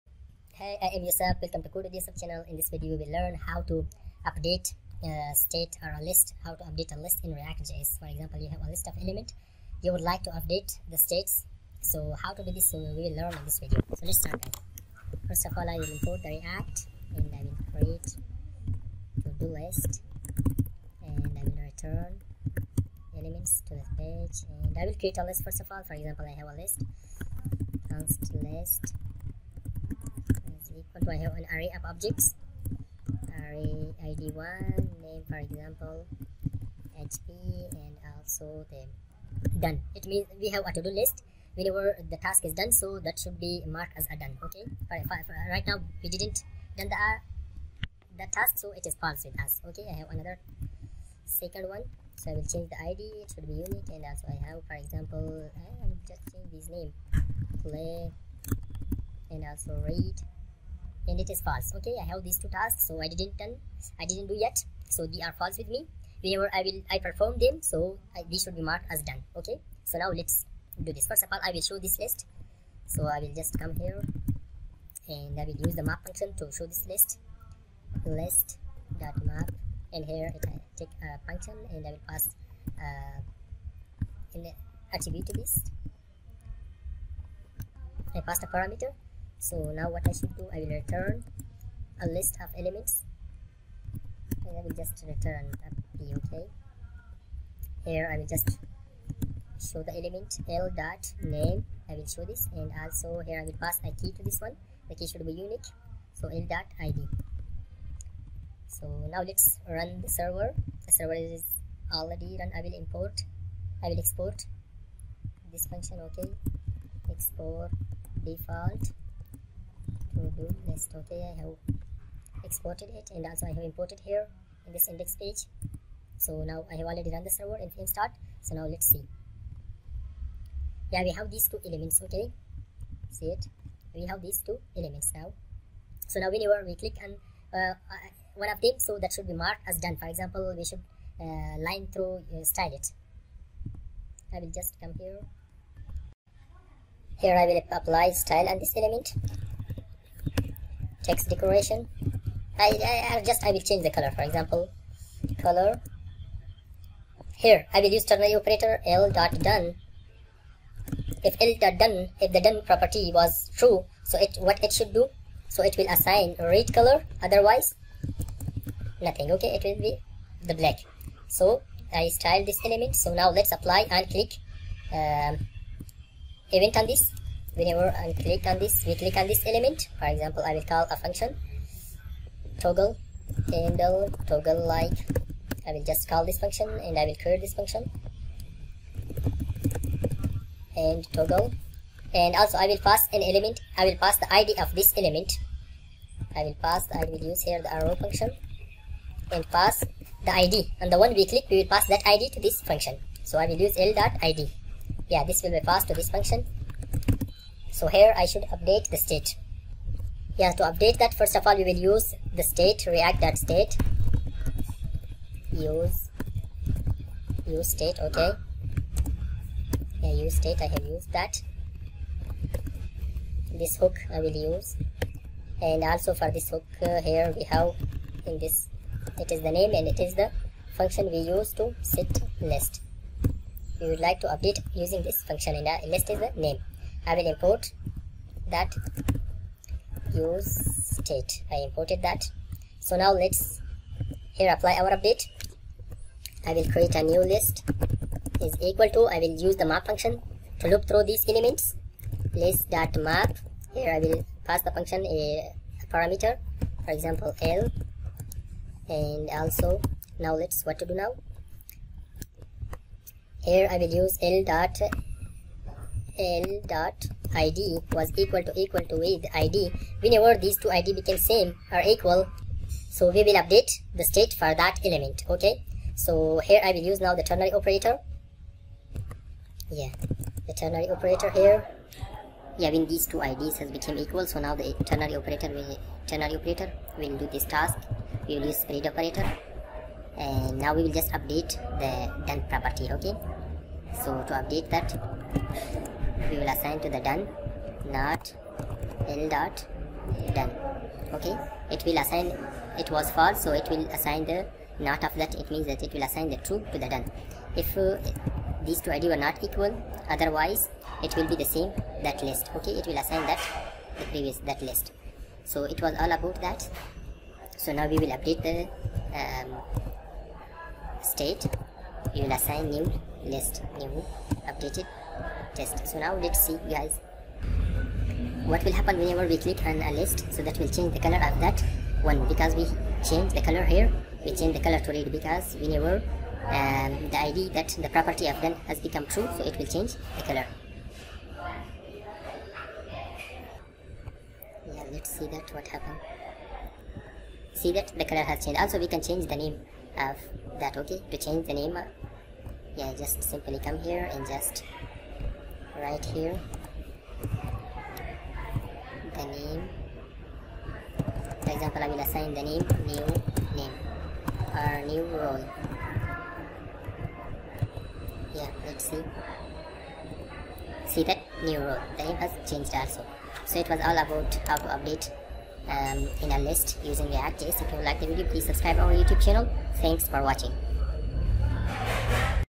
How to Update an Element in React JS | Update List in React JS
In this video we will create a program for how to update an element from list in react js. updating an element from react js list is very simple and we explain in a few line of code.